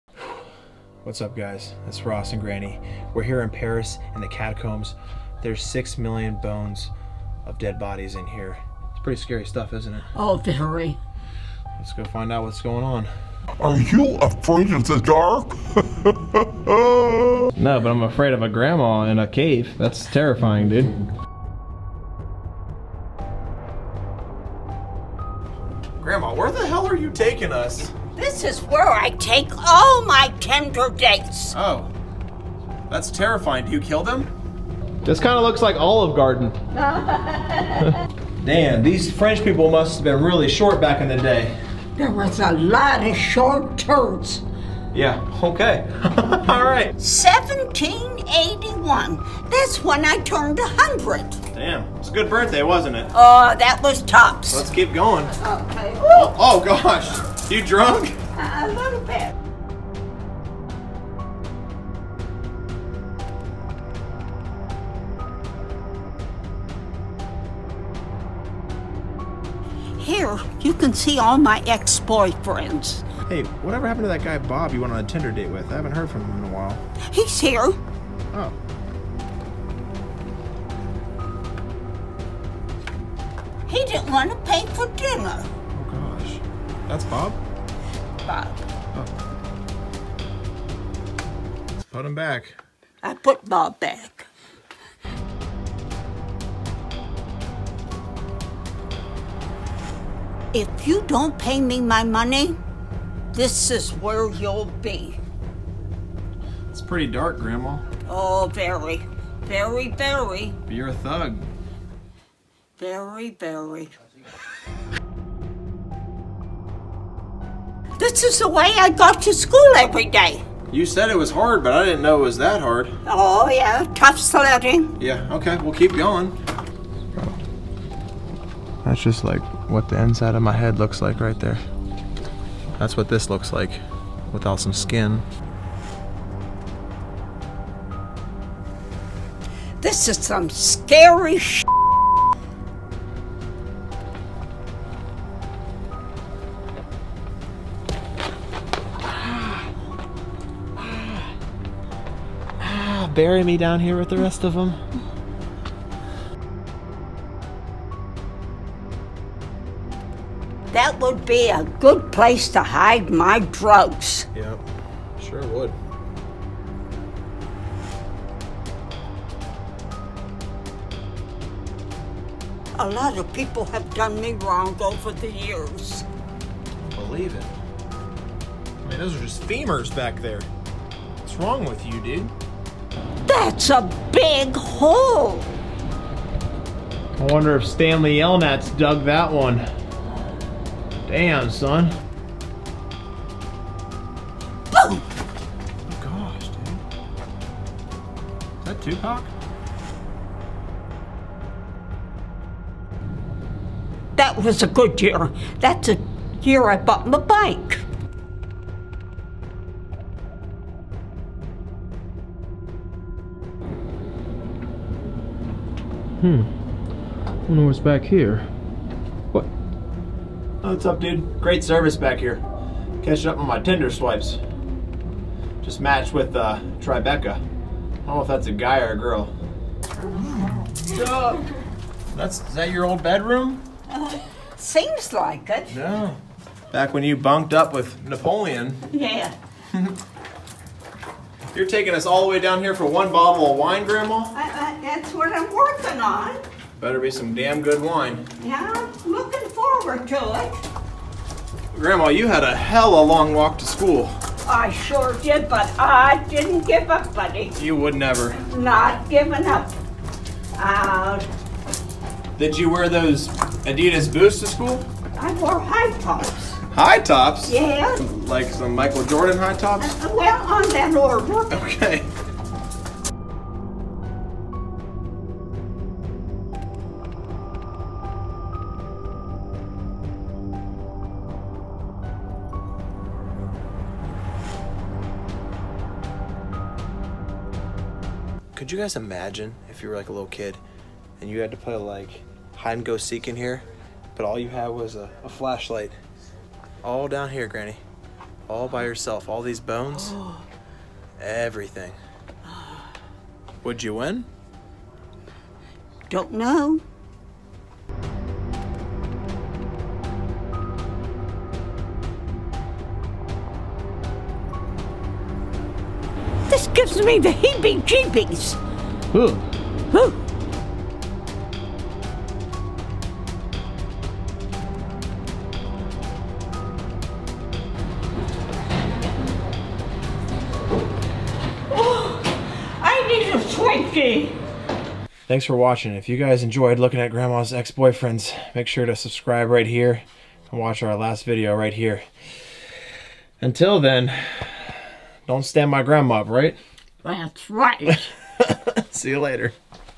Grandma Finds Hidden Graveyard | Ross Smith
You won't believe what grandma finds in the graveyard and the memories they brought back for her.